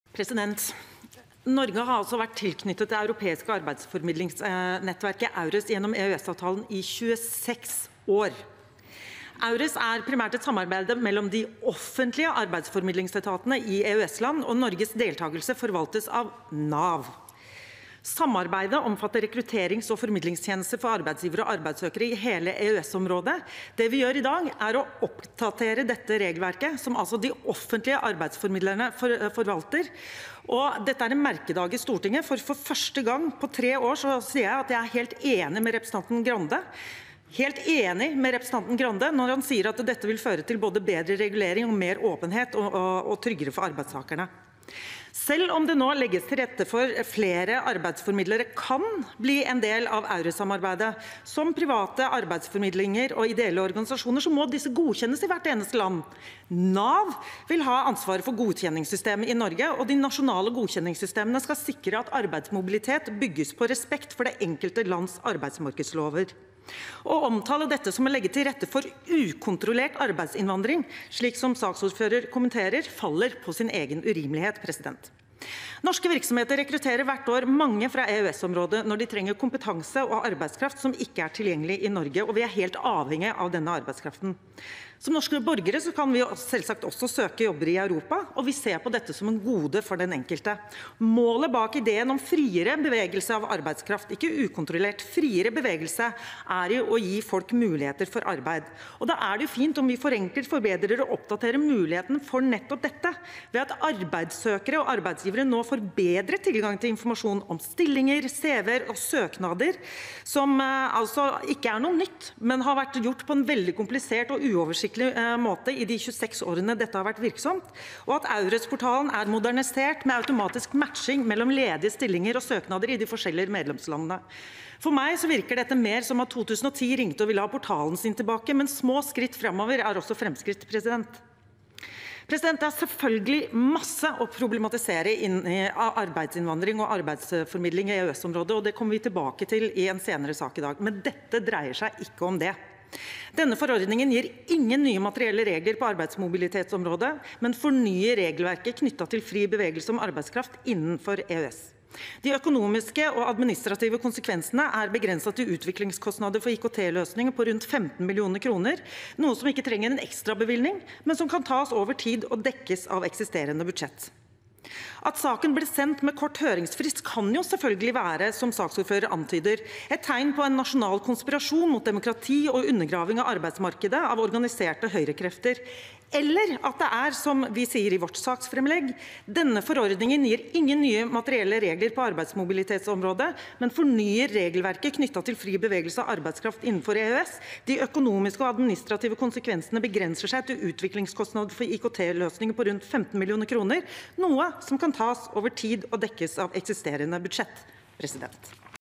08.12.2020 Sak 8 1 Heidi Nordby Lunde
Om Endringer i lov 14. desember 2012 nr. 81 om gjennomføring i norsk rett av EØS-avtalen vedlegg V punkt 2 (forordning (EU) nr. 492/2011) om fri rørsle av arbeidstakarar innanfor EØS-området (EØS-arbeidstakarlova) (Innst. 108 L (2020–2021), jf. Prop. 141 LS (2019–2020))